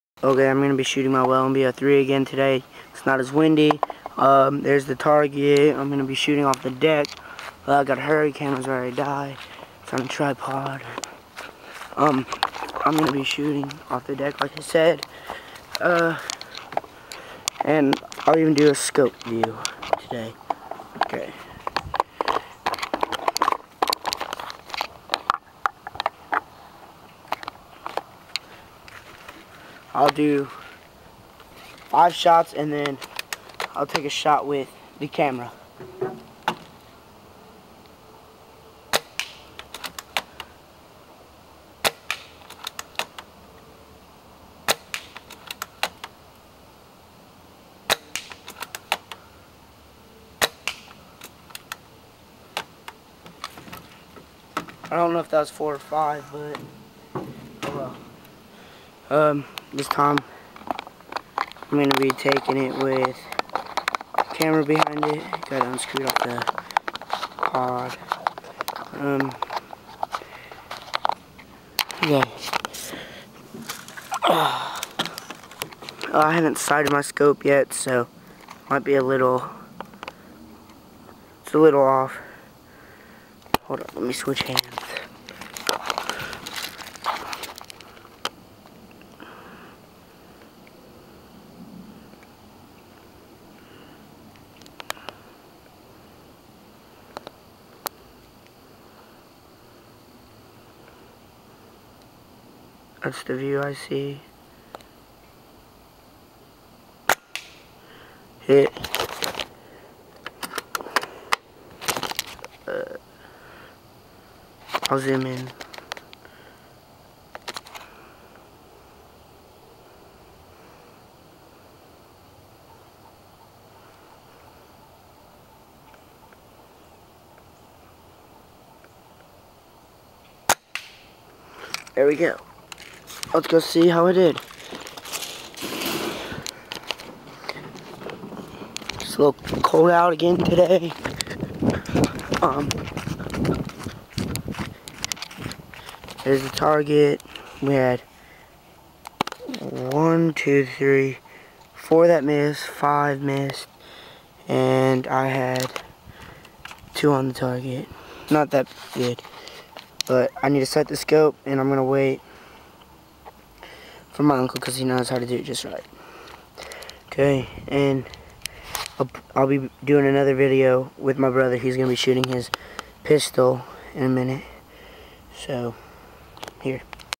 well mb03 shooting at new target
shooting at target not as windy cause it was a couple days later i will have a war up this weekend probably